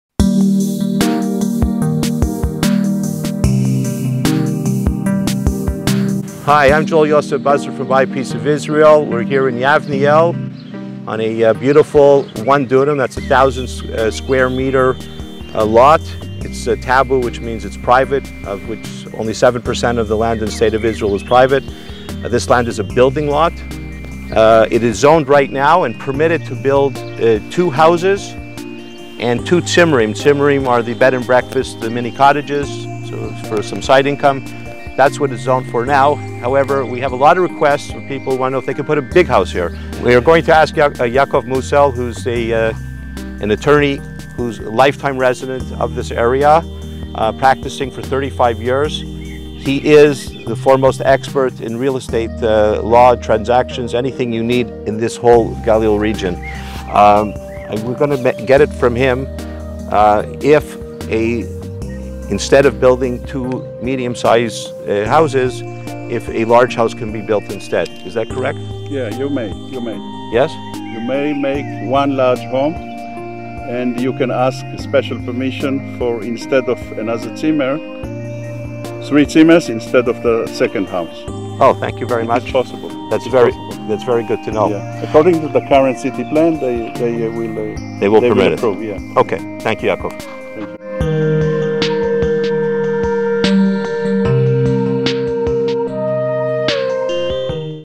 Cheapest Real Estate in Israel - A great property in the Lower Galilee
A great residential private lot in the Lower Galilee village of Yavniel
Great views, quiet country setting
8 minutes from Lake Kinneret
Zoned for 2 medium size houses and 2 bed and breakfast units, or 1 large house and 3 bed and breakfast units.

1 dunam (1/4 acre) building lot (housing)
Yavniel, Lower Galilee
Asking Price: 1,300,000 shekels (apx. $420,000)